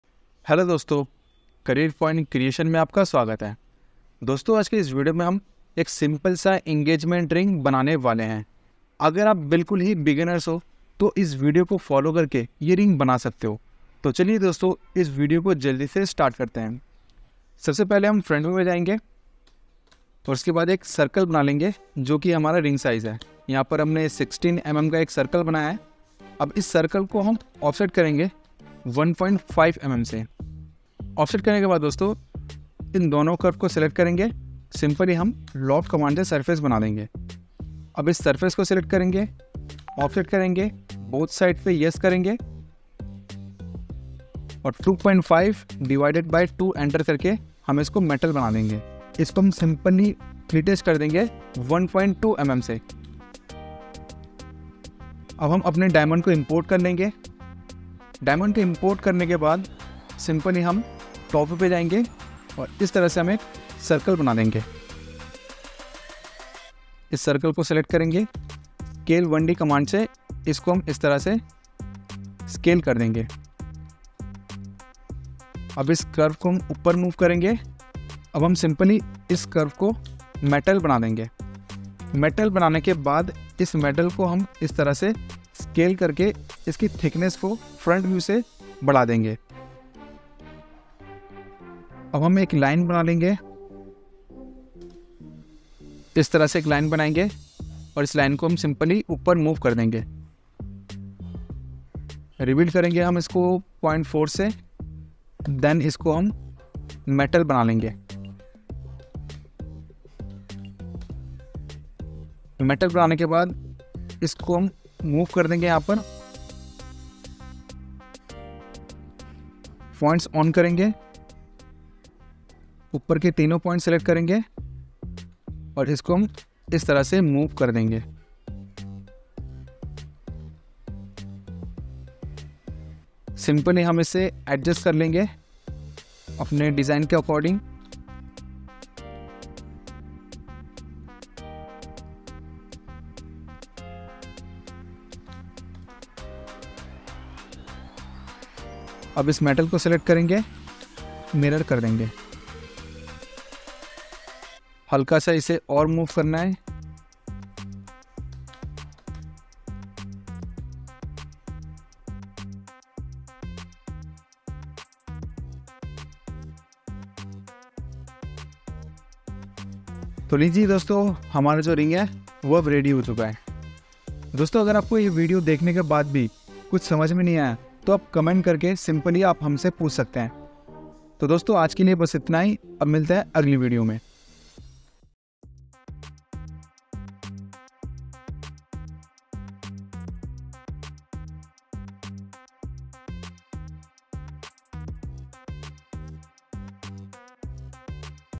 Designing the Perfect Engagement Ring in CAD | Rhino Tutorial for Beginners
CAD Jewelry Design Course For More info about this course fill out the form.